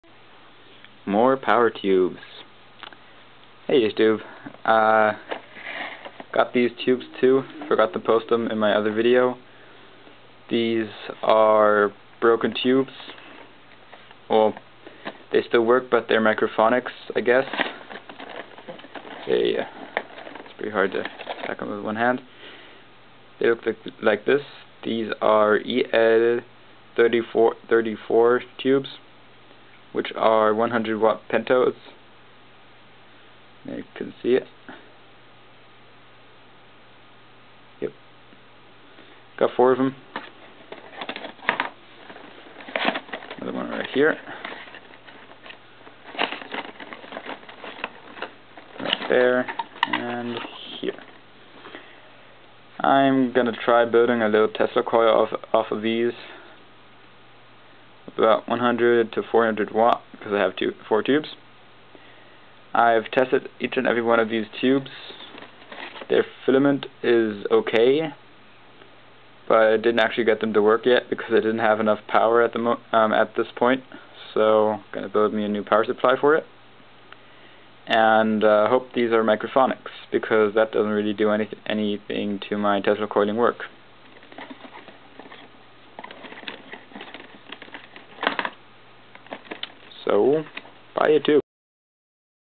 Power Tube Colection Part 2
i forgot my other tubes wich are EL34s.